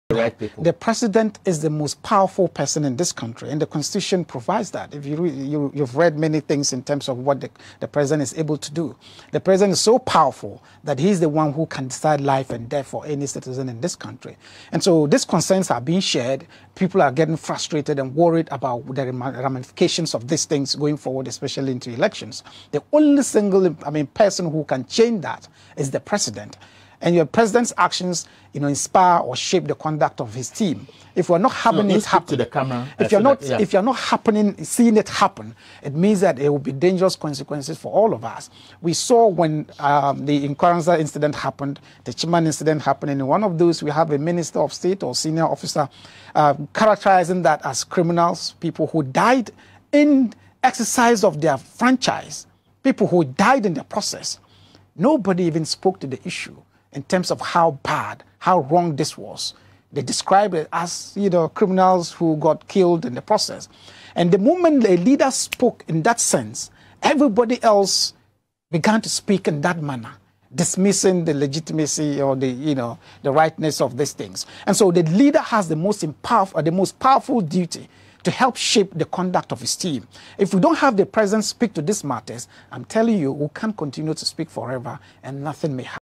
Counter extremism expert warns Akufo-Addo about terrorists in Ghana
President Akufo-Addo must address the country on curbing terrorism – Mutaru  Muqthar, Exec. Director of Africa Centre for Counter Extremism